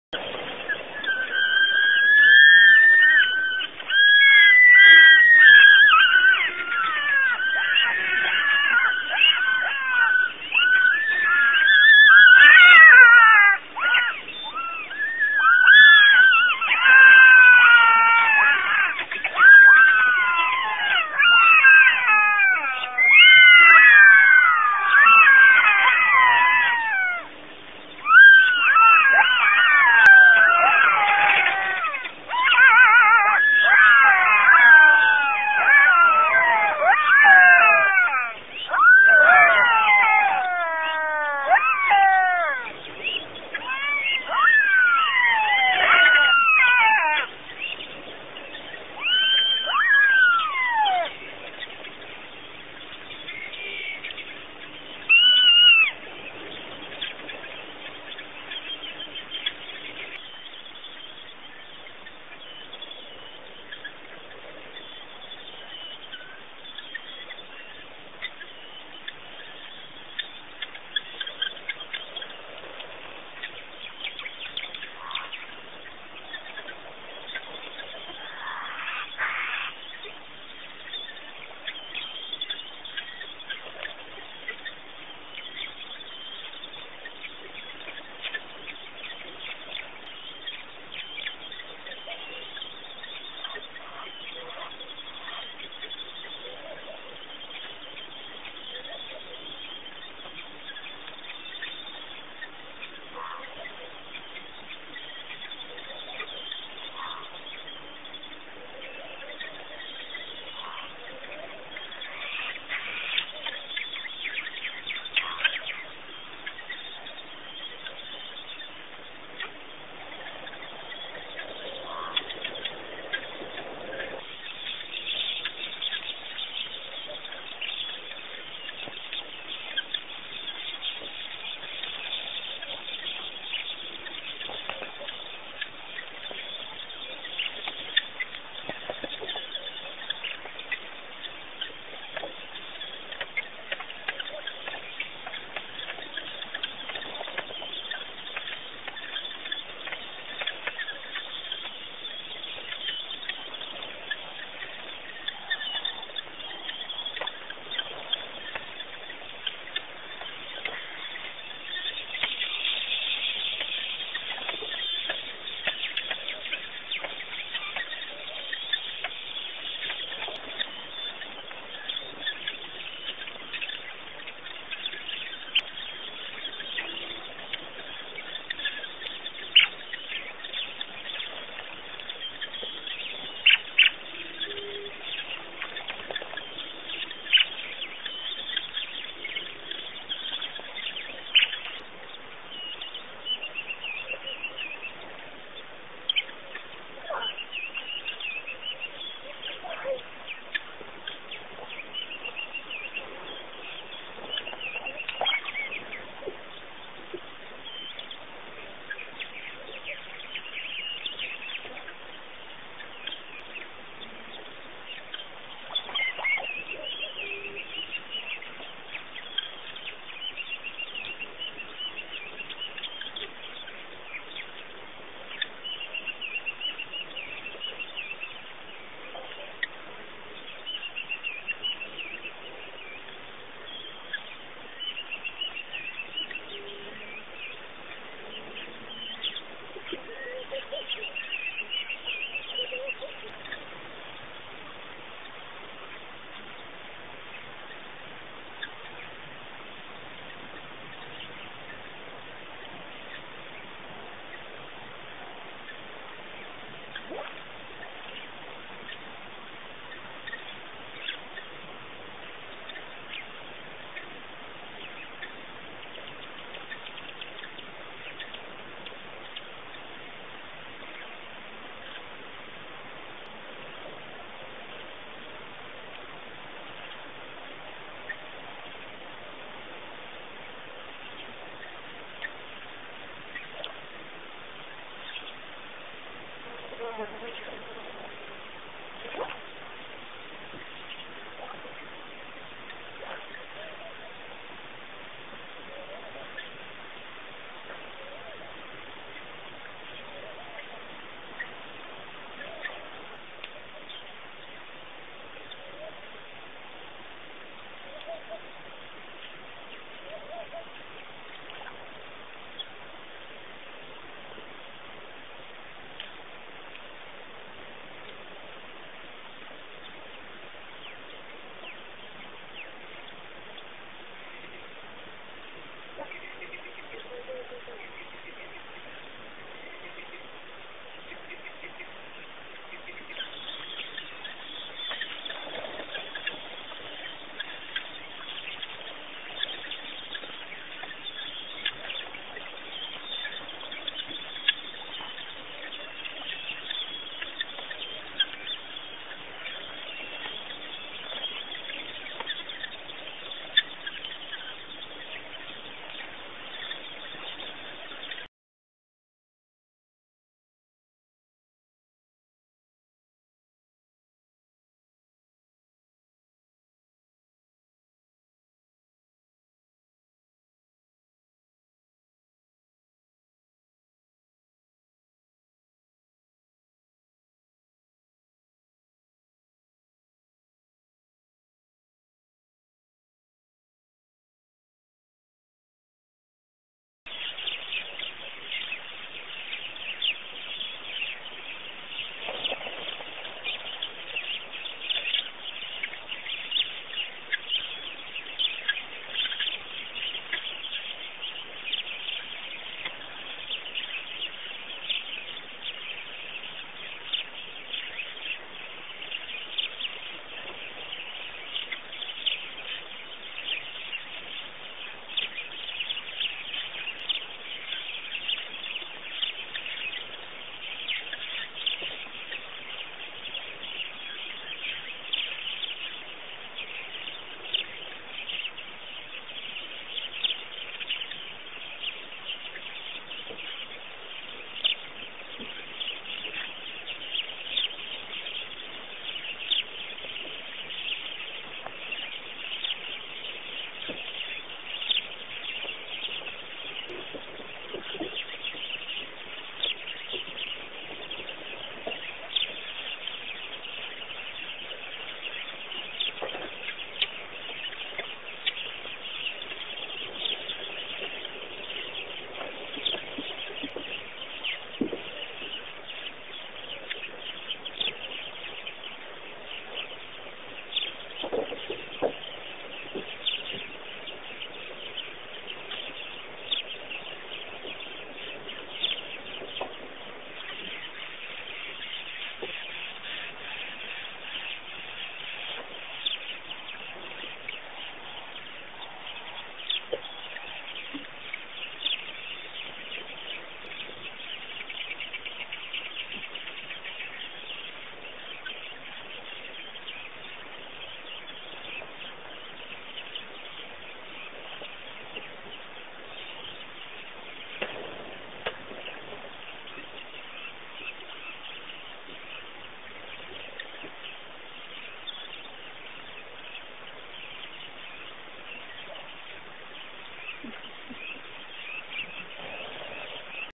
Birds! Birds! Birds! 50
Little Grebe, Little Egret, Pied Wagtail, Masked Weaver, Tawny Eagle, Black Flycatcher, African Fish Eagle. Sightings from Pete's Pond & WildEarth Game Drives. Begins with jackals greeting the day at PP & watch for Fatty the Nile Croc trying for the Grebe!